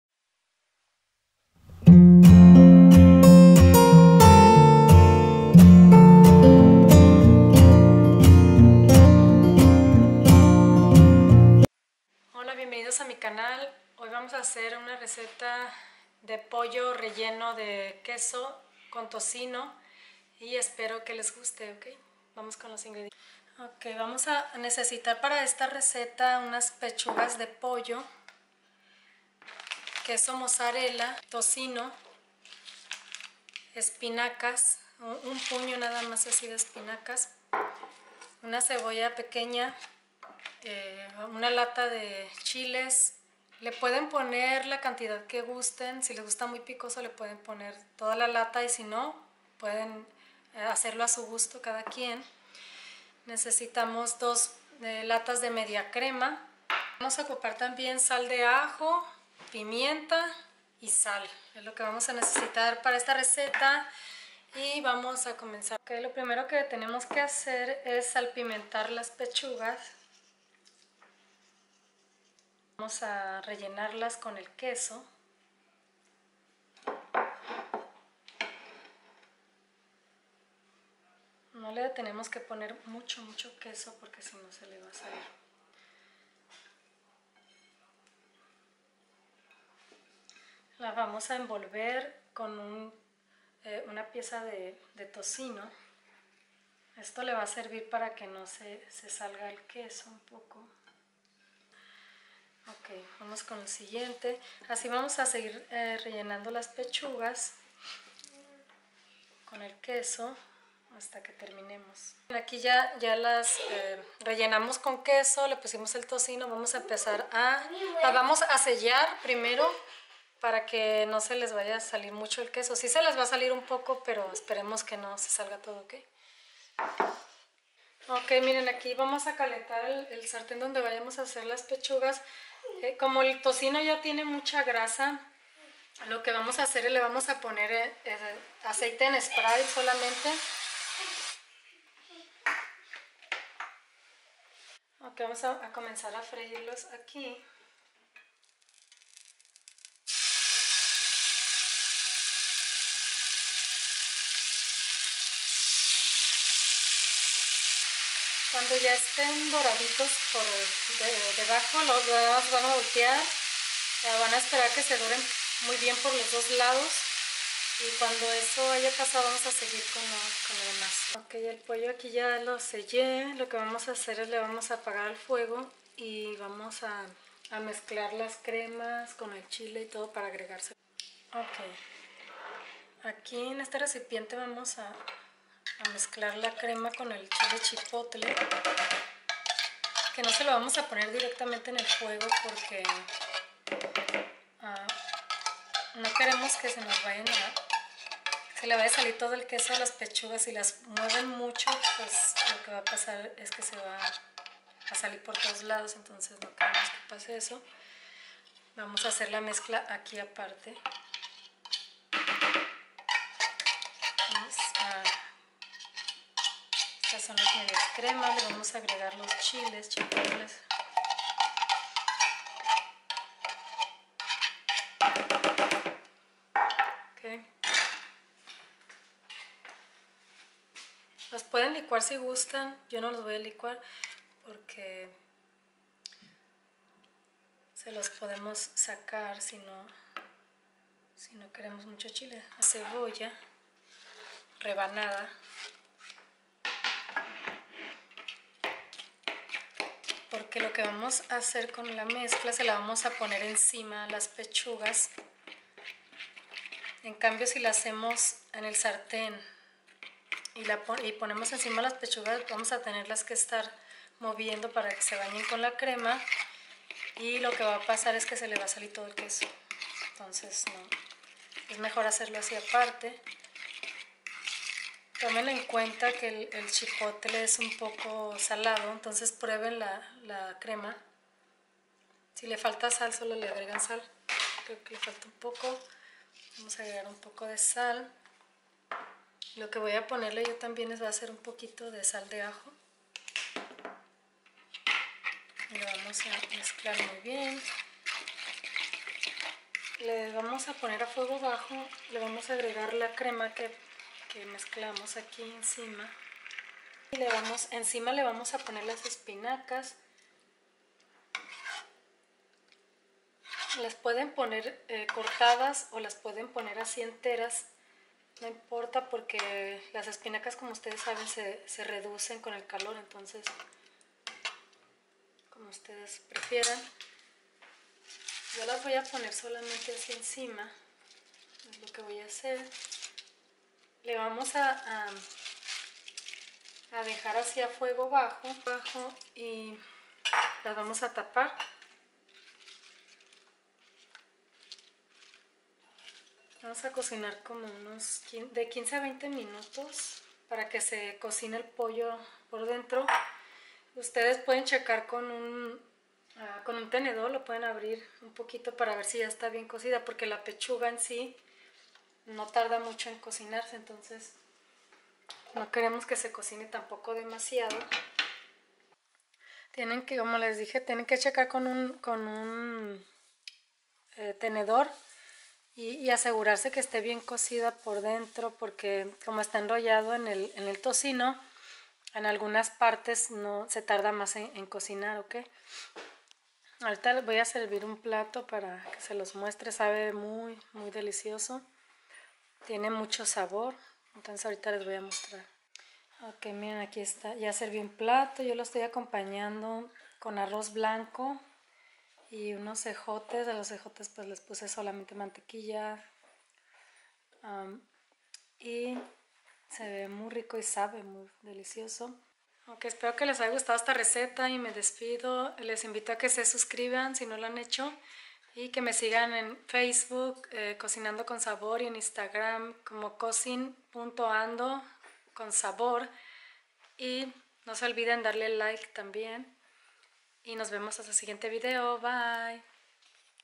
PECHUGAS RELLENAS DE QUESO/CON TOCINO Y ESPINACAS
Estas pechugas están rellenas con queso ,envueltas con tocino y con un poco de espinacas.   INGREDIENTES:Pechugas de pollo, tocino,queso(el necesario y el de su gusto),1 manojo de espinacas,2 media crema,1/2 lata de chiles chipotles,sal,pimienta,sal de ajo.